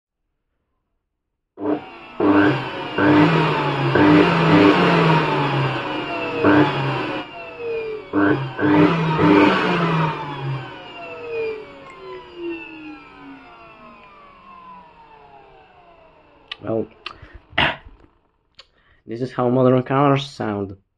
When a vacuum cleaner sounds like a modern I4 Engine, or opposite.
Yes. You can rev your vacuum cleaner.